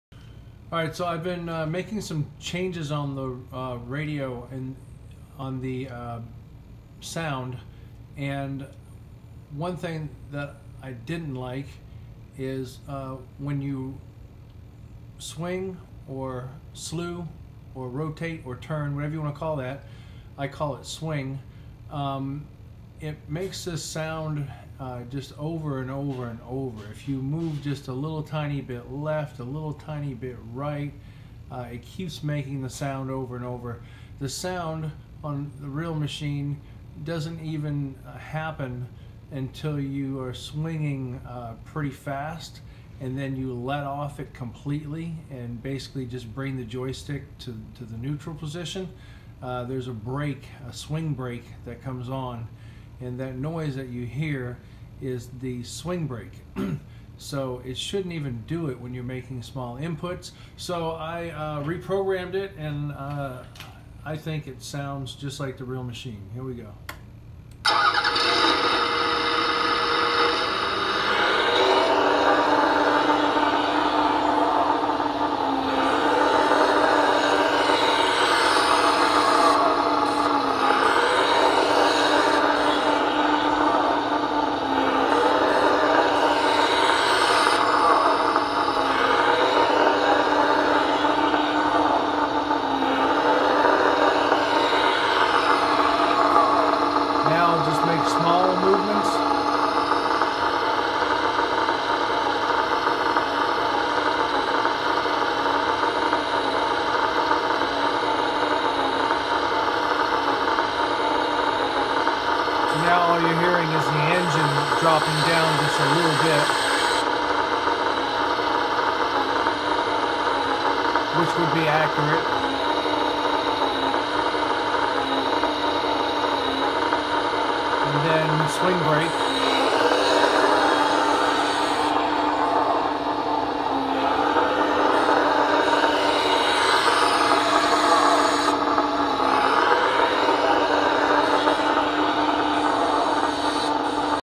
Servonaut sound program fix
didn’t sound right was making a sound when it wouldn’t on a 1:1 machine so I fixed it.